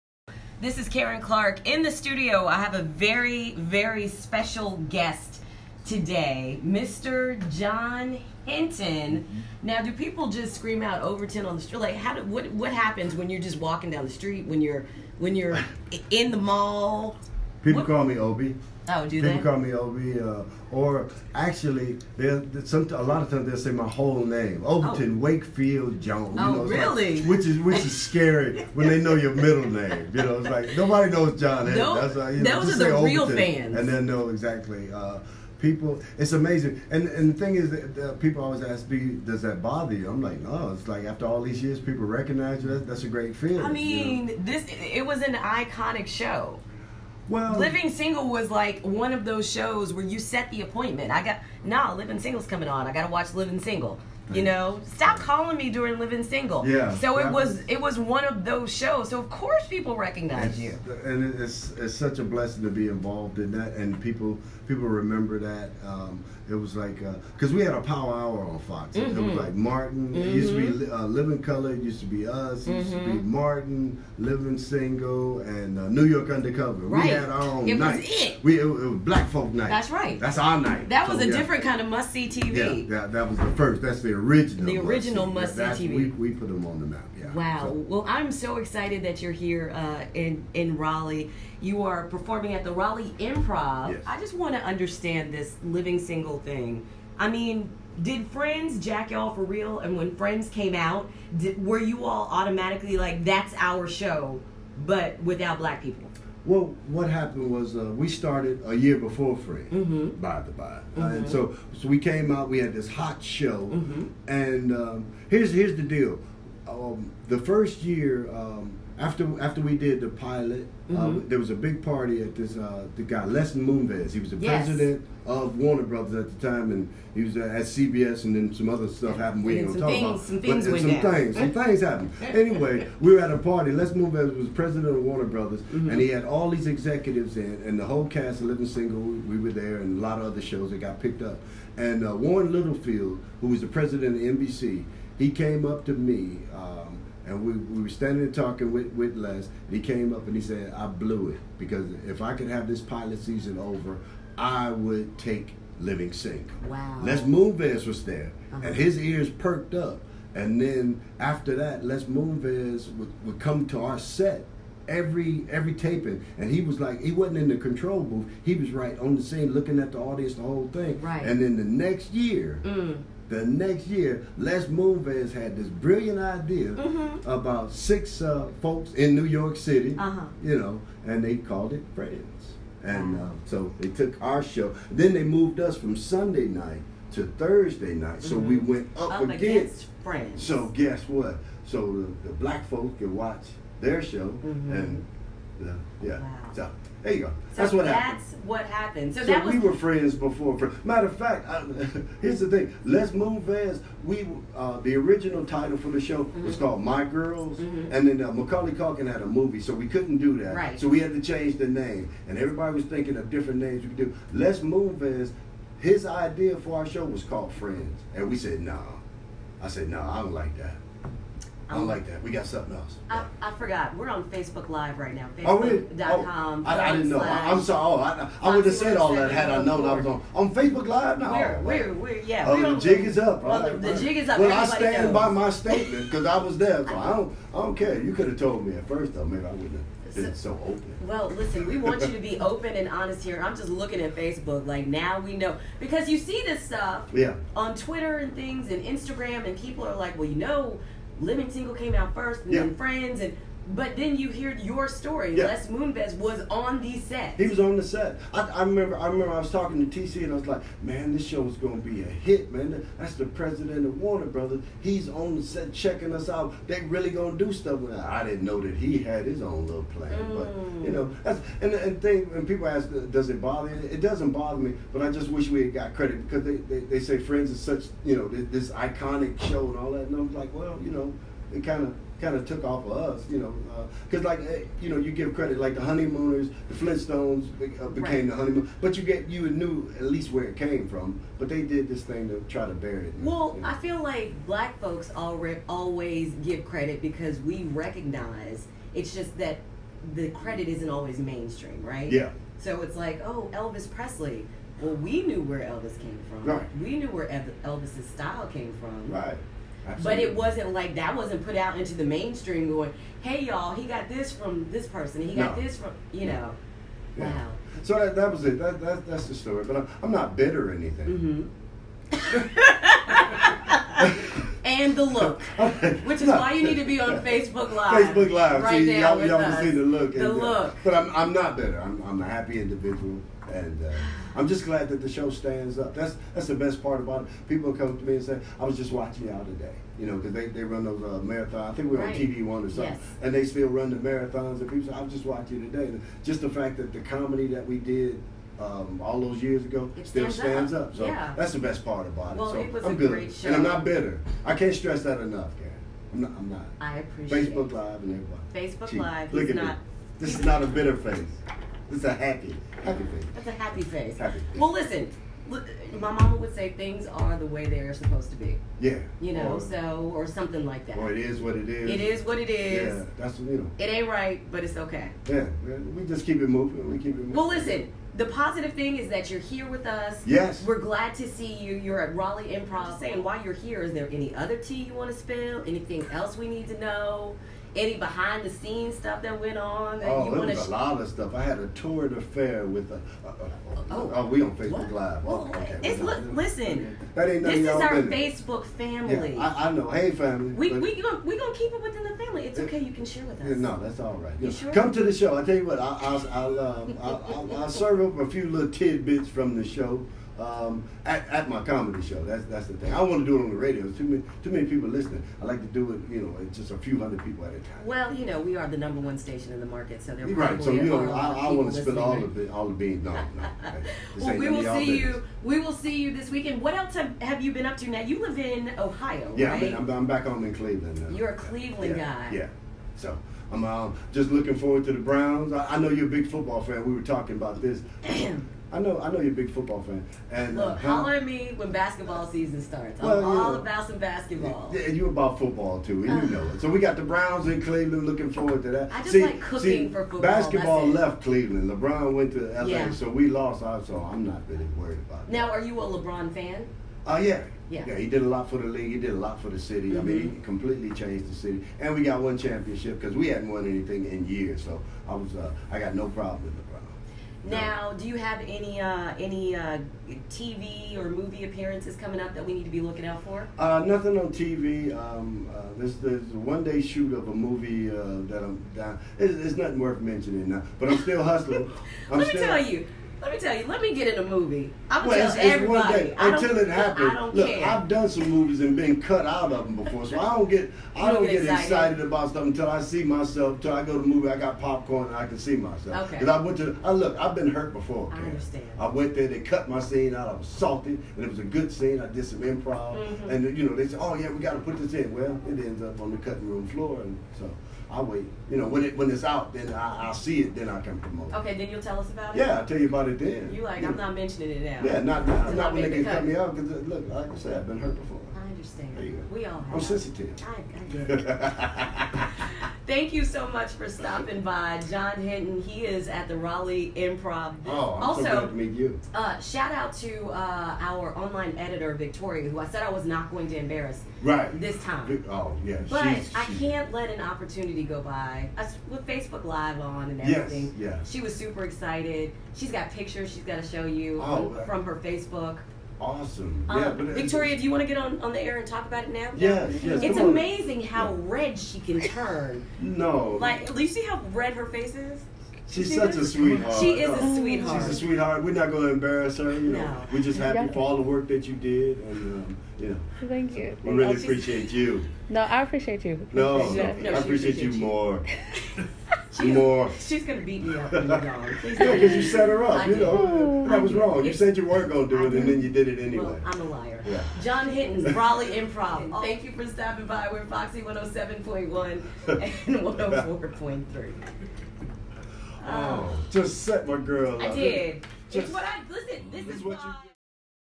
Living Singles Overton 'Obie' Wakefield Jones - John Henton
John Henton, we know him best as Overton 'Obie' Wakefield Jones on Living Single tells all with Karen Clark. He let the Foxy family know all the details on what came first Living Single or Friends.